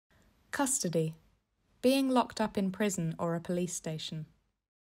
Custody: a definition from Legal Choices
An Easy Read definition of "custody" from Legal Choices.

For more definitions of legal terms, see our law dictionary